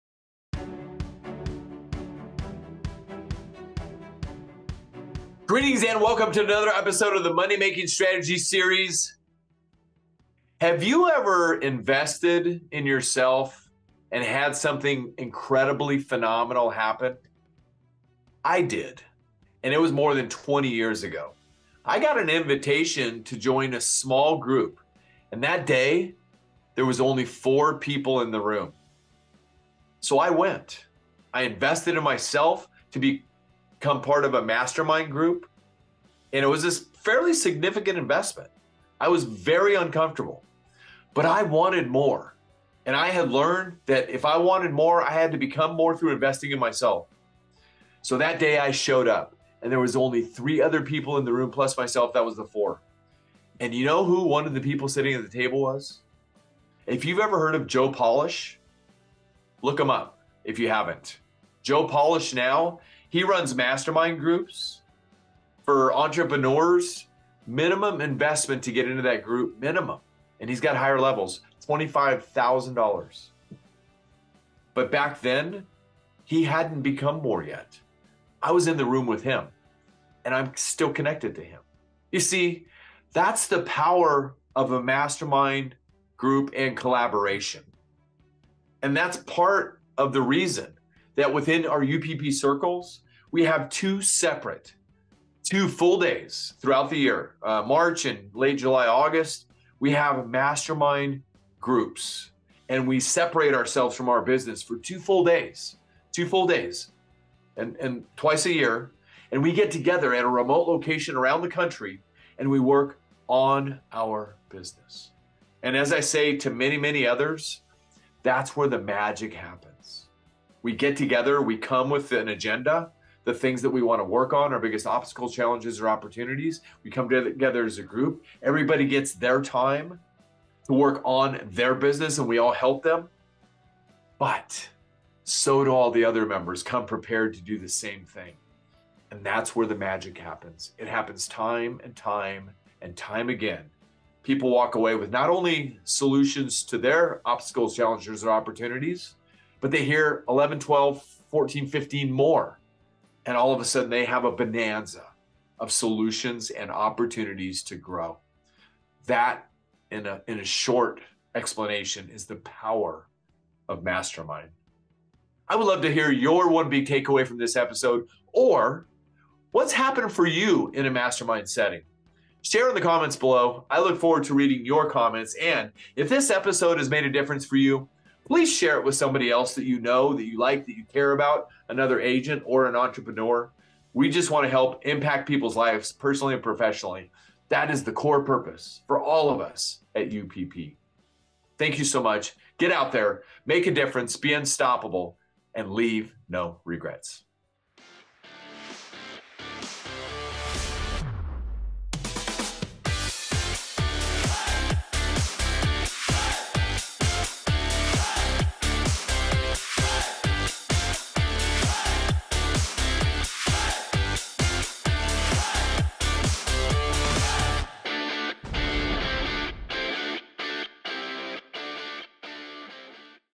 [MMS] ✏️ Power of Collaboration
🎥 Join us as we explore how Independent Insurance Agency can benefit from the collective knowledge and experience of a group of peers in a Mastermind setting. We'll share real-world stories of how collaboration can lead to breakthrough ideas, new business opportunities, and a more robust network of like-minded professionals.
Whether you're a seasoned Independent Insurance Agency or just starting out, this episode is packed with actionable insights and practical tips to help you succeed in your agency. So don't miss out on the Power of Collaboration.👊